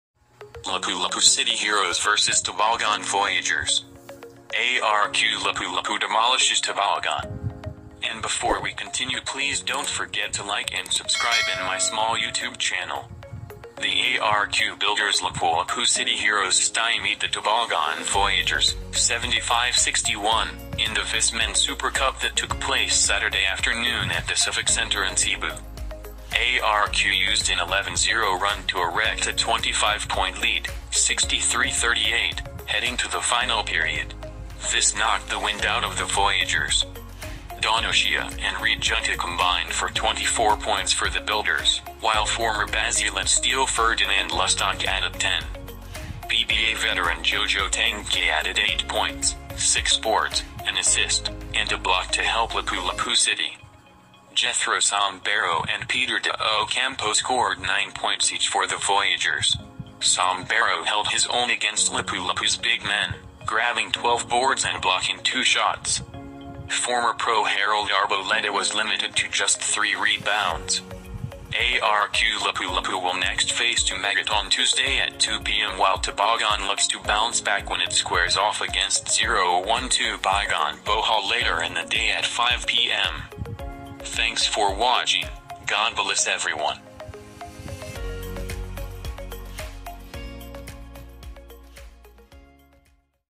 VisMin Super Cup | Lapu-Lapu City Heroes Vs. Tabogon Voyagers | ARQ Lapu-Lapu Demolished Tabogon
A.R.Q used an 11-0 run to erect a 25-point lead, 63-38, heading to the final period. This knocked the wind out of the Voyagers.

Dawn Ochea and Reed Juntilla combined for 24 points for the Builders, while former Basilan Steel Ferdinand Lusdoc added 10

PBA veteran Jojo Tangkay added eight points, six boards, an assist, and a block to help Lapu-Lapu City.

Jethro Sombero and Peter de Ocampo scored nine points each for the Voyagers. Sombero held his own against Lapu-Lapu’s big men, grabbing 12 boards and blocking two shots.

Former pro Harold Arboleda was limited to just three rebounds.